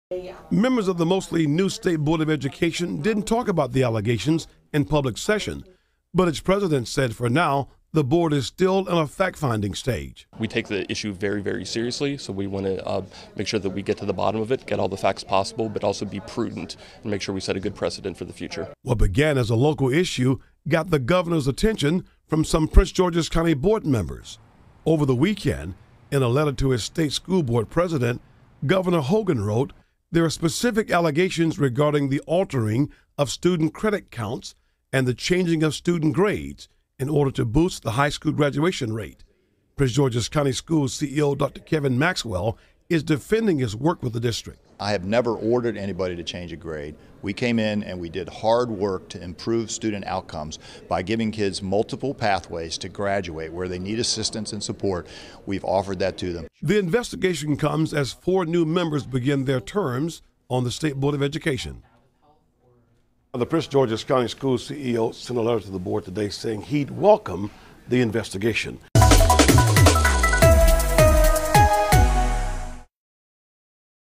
Video: Third party to investigate Prince George's County schools
The Maryland State Board of Education will turn to a third party to conduct an investigation into allegations of grade fixing at the highest level of the Prince George's County Public Schools. The outside organization, yet to be named, will investigate whether Prince George's County school officials fixed grades in order to help more students graduate.